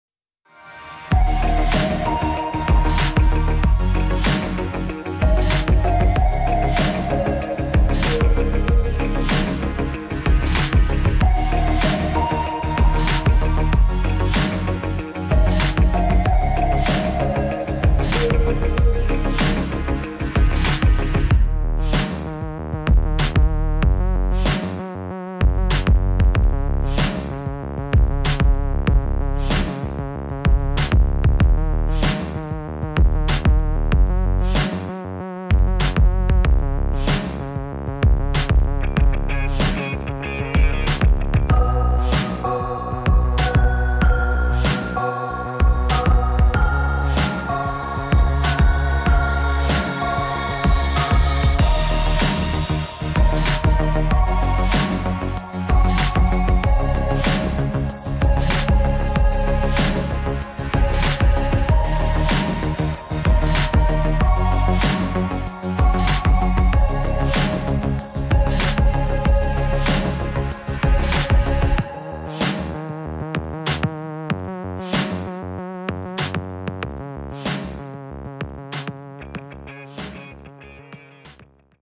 Paris prepare: in slow motion
See Paris Saint-Germain put through their paces as they prepare for their UEFA Women's Champions League final against 1. FFC Frankfurt in Berlin.